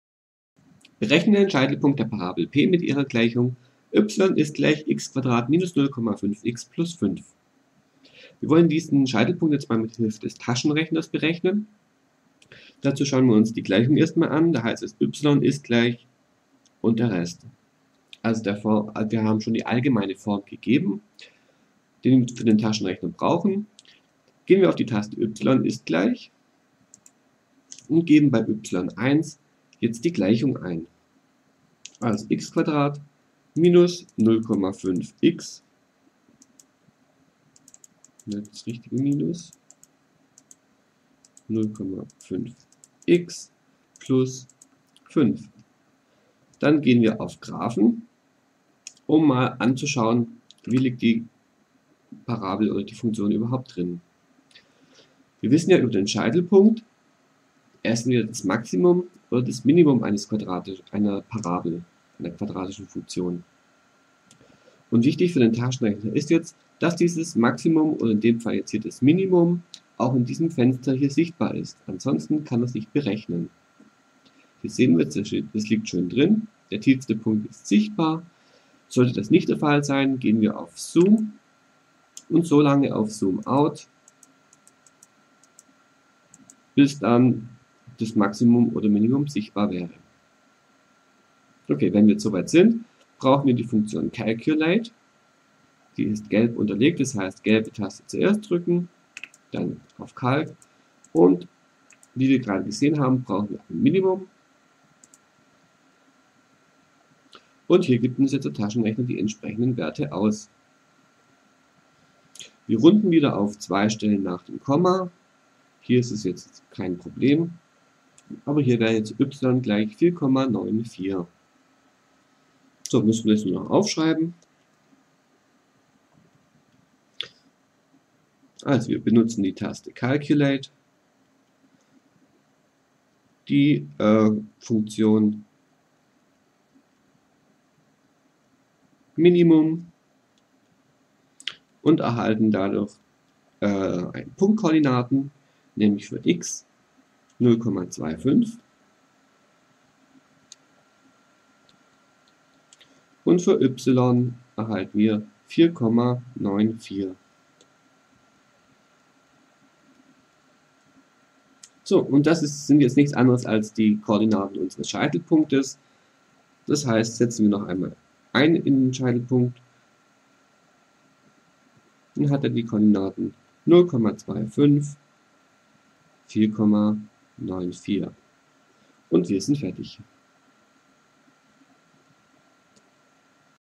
Scheitelpunkt - Berechnen aus Allgemeiner Form (CALC-MaxMin) (9I.3 | 10II.1)
Wie berechnet man den Scheitelpunkt einer Parabel, deren Funktionsgleichung in allgemeiner Form gegeben ist.
- über die Funktion [CALC] - Maximum/Minimum
GTR: SHARP EL 9900

Realschule Bayern
Klasse 10 II/III